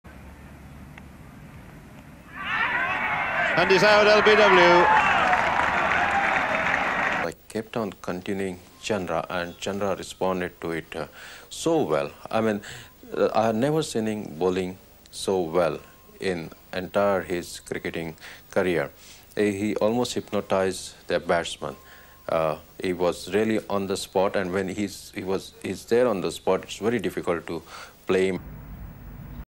Late Ajit Wadekar on Bhagwat Chandrashekhar's 6/38 against England in The Oval
India's captain, Ajit Wadekar was confident of victory - "I went to sleep when I came back to the dressing room after being dismissed. I was nudged awake by Ken Barrington, the England manager, who told me that we had won. I said to him that I always knew we'd win"